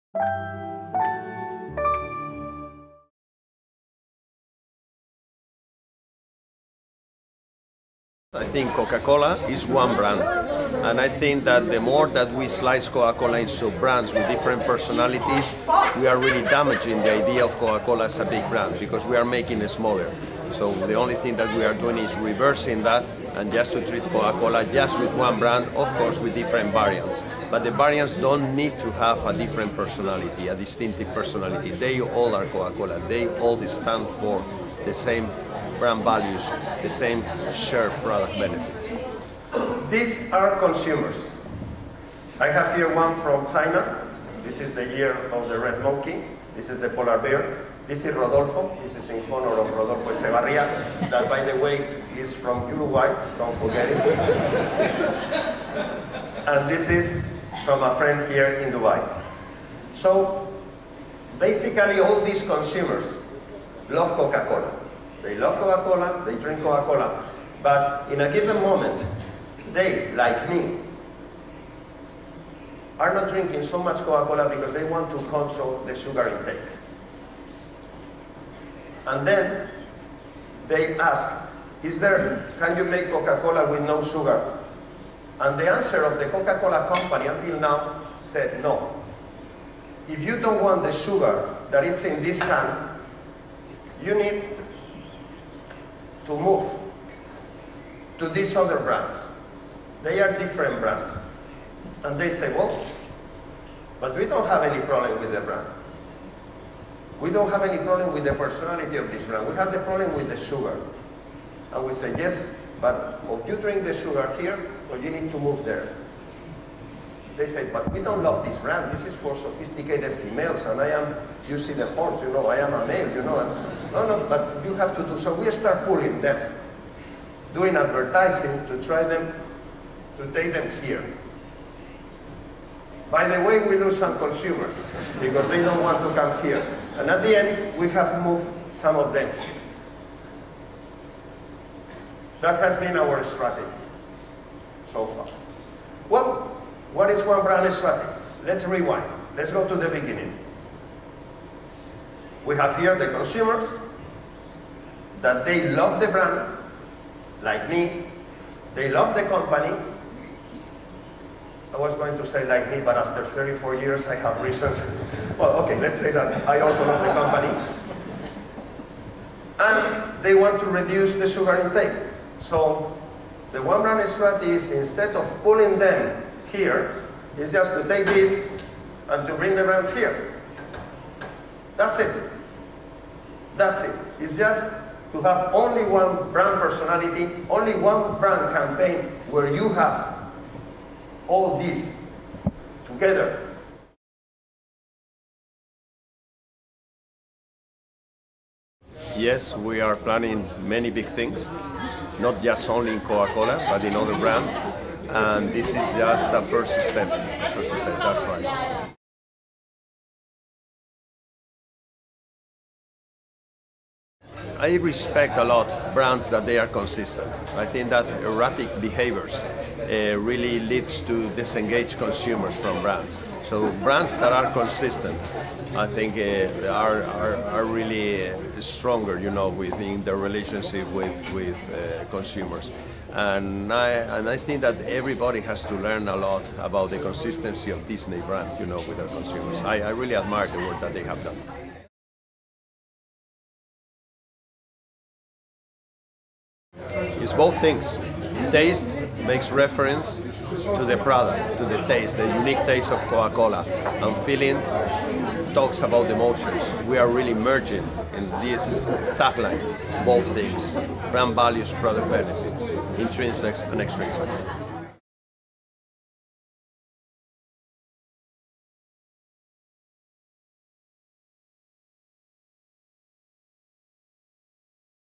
Why the iconic Coca-Cola brand has taken its cue from Disney…
Last week, Coca-Cola’s Chief Marketing Officer and Executive Vice President Marcos de Quinto unveiled the company’s new ‘One Brand’ global marketing strategy at a glitzy event held in Dubai. The bold new campaign, with the tagline ‘Taste the Feeling’, unites Coca-Cola, Coca-Cola Light/Diet Coca-Cola and Coca-Cola Zero under the iconic Coca-Cola brand.

“We are planning many big things, not just within Coca-Cola, but within the other [company] brands, and this is just a first step,” de Quinto explained to Adlip.

During his presentation, de Quinto made reference to Disney and praised the success and longevity of the much-loved brand.

“I really admire the work that [Disney] have done,” adds de Quinto, pointing out how consistent and steady the brand has remained over the years. He notes that erratic behaviour really leads to ‘disengaged consumers’ for brands.

He highlights the importance of keeping Coca-Cola one brand, and not slicing it into too many different parts. That said, the various brands within the Coca-Cola stable ‘must share the same values and benefits’, says de Quinto.

Taste the Feeling…

“’Taste’ makes reference to the unique taste of Coca-Cola, and ‘feeling’ talks to the emotion – so with this tagline we are merging both things - brand values and product benefits,” explains de Quinto.